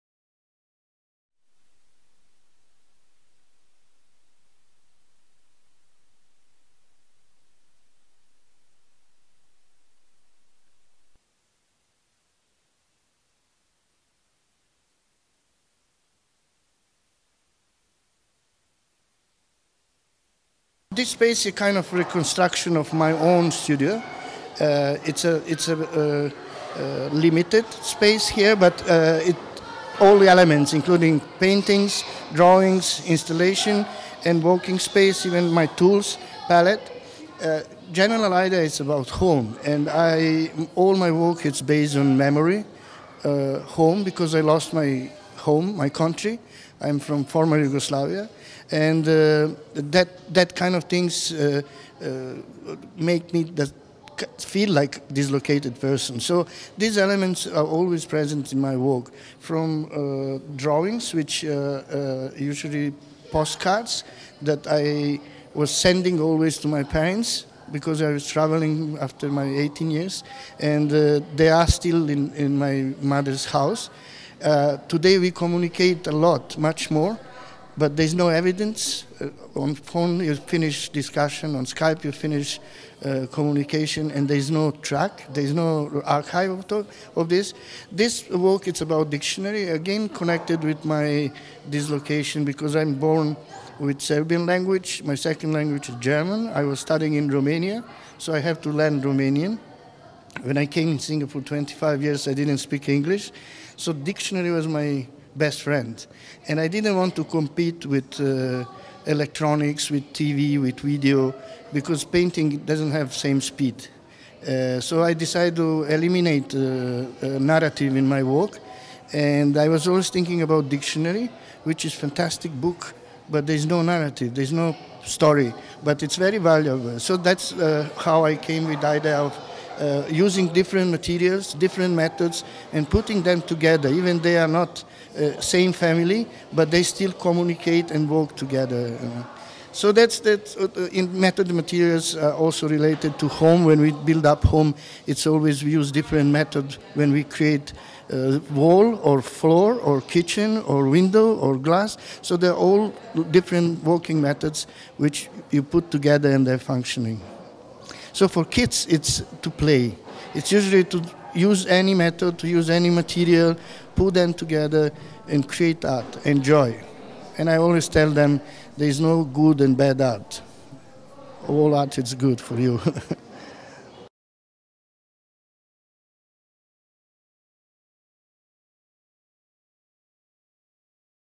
Art Is Child’s Play: Milenko (The Keppel Centre For Art Education)
A sneak peak at The Keppel Art Education Centre. It’s set to open in November.

SIX-SIX NEWS: News & Views on trending issues in Singapore & beyond.